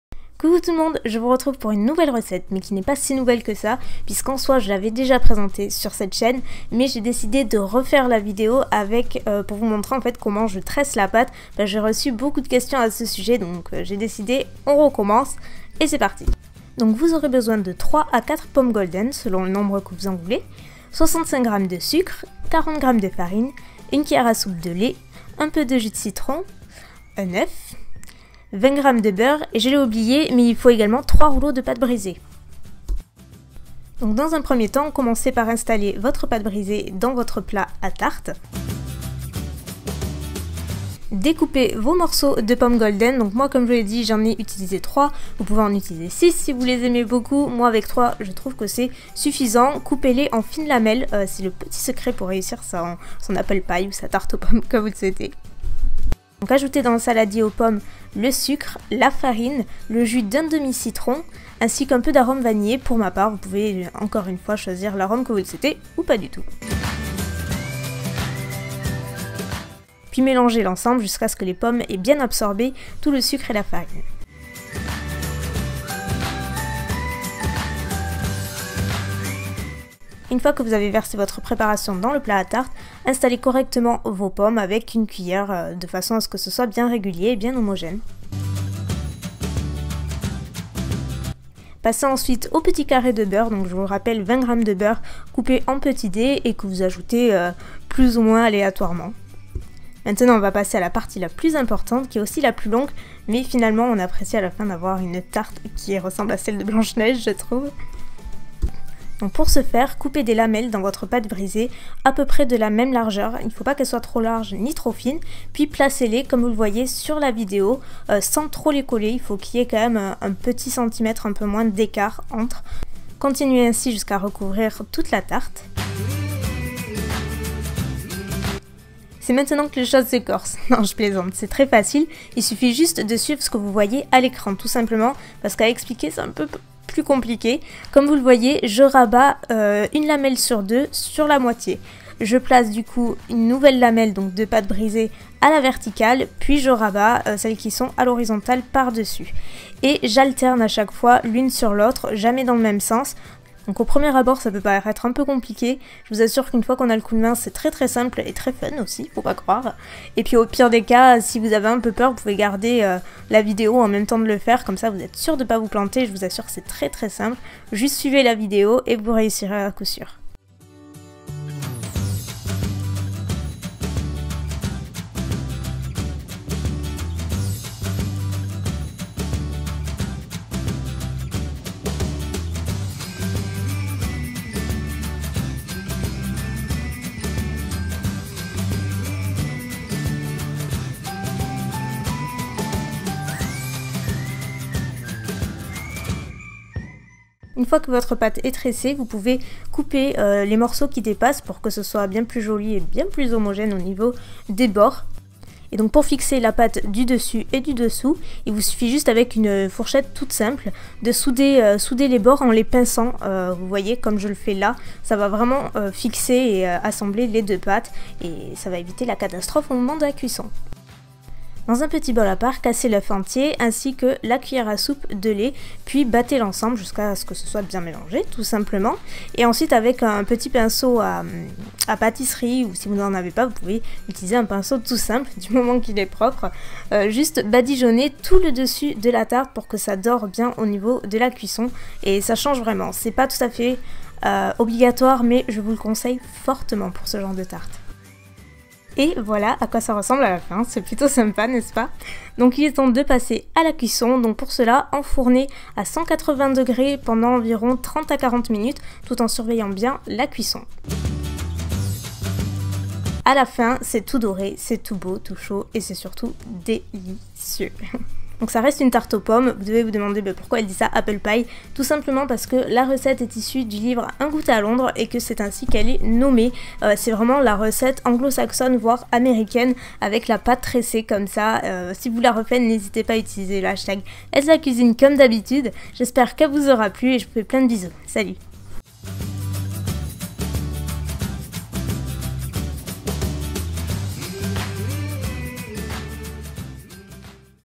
♡ Recette : Apple pie
♡ Ingrédients :
- 3 pâtes brisées
- 65g de sucre
- 40g de farine
- 1/2 jus de citron
- 3-5 pommes
- 20g de beurre
- 1 oeuf
- 1 c.a.s de Lait

♡ Ce que je porte  :
Vernis : Lady - Dior